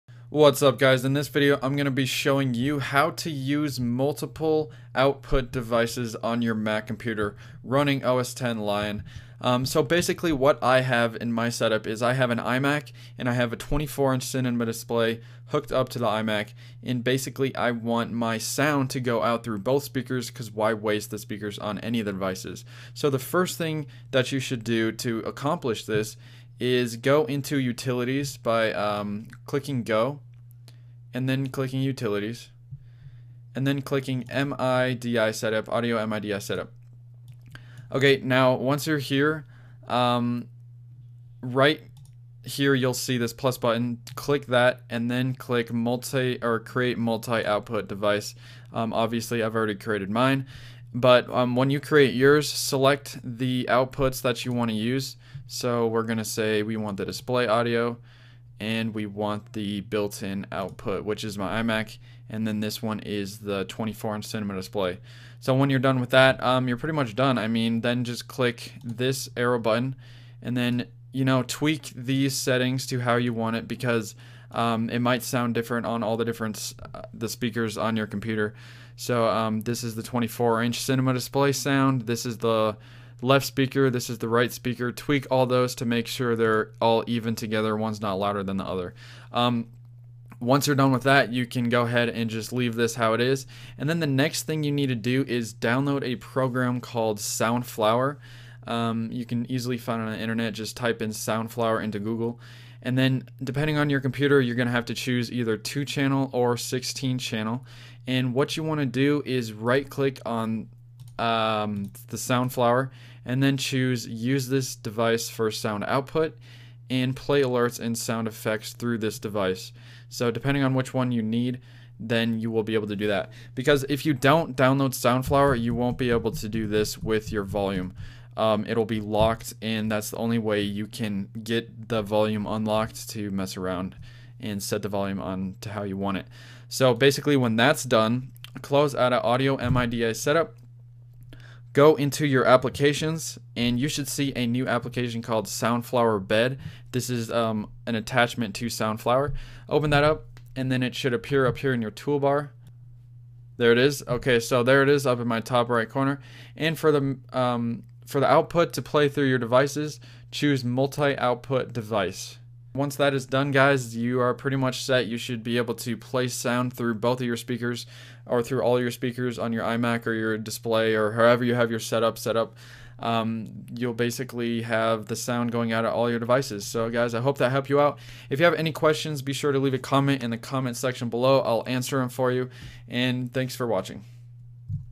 Sound Output on Multiple Monitors on a Mac
This video shows you how to easily setup your Mac to play sound through all of the speakers that are connected to the computer. (For example, I have an Apple Cinema Display connected to my iMac and I want the sound to play out of the speakers on the Cinema Display and the iMac simultaneously.)